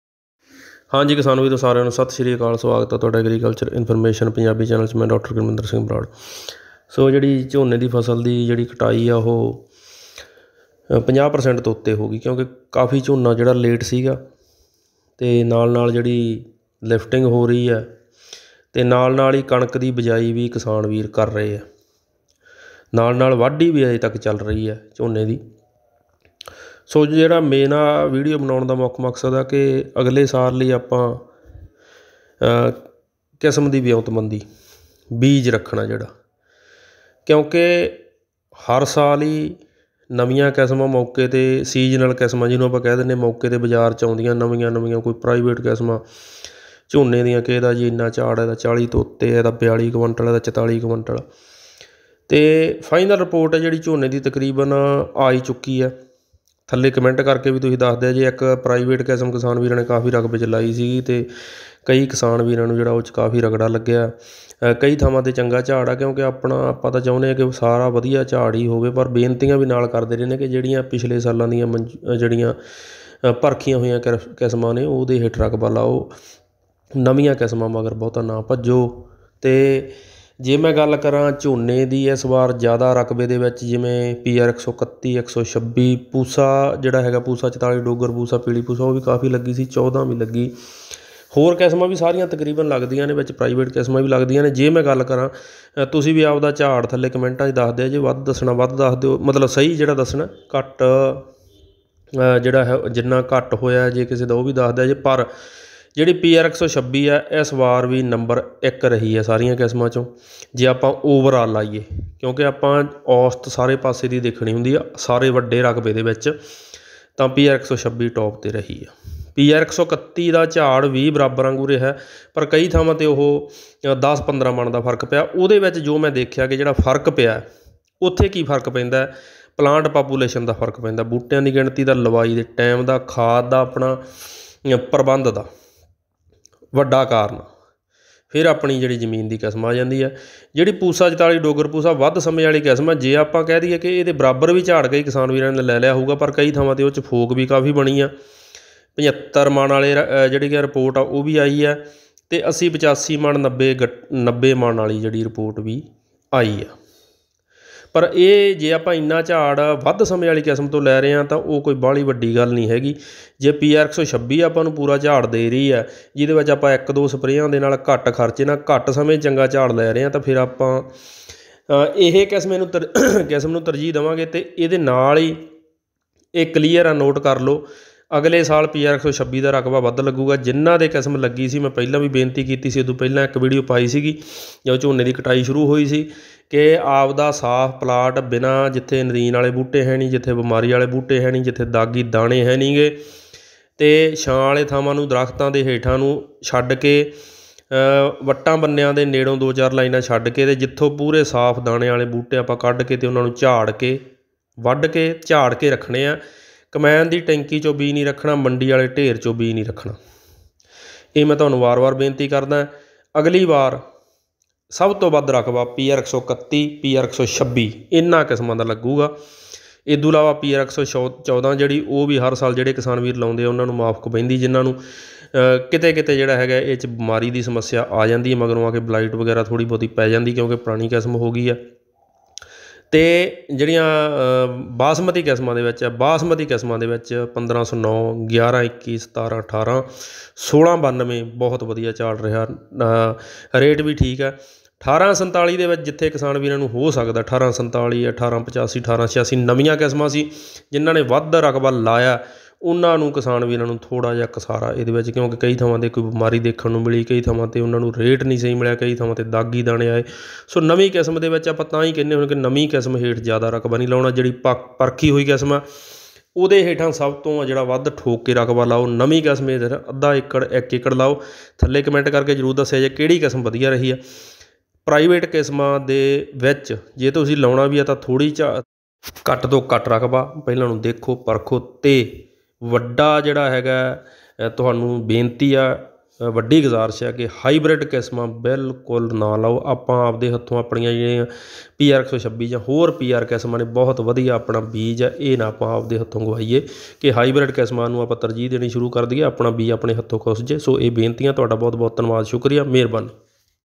ਝੋਨੇ ਦੇ ਝਾੜ ਦੀ ਫਾਈਨਲ ਰਿਪੋਰਟ, ਇਸ ਕਿਸਮ ਹੇਠ ਵਧੇਗਾ ਰਕਬਾ, ਨੰਬਰ 1 | Paddy yield final report
ਆਪਣਾ ਬੀਜ ਝਾੜ ਕੇ ਜਰੂਰ ਰੱਖੋ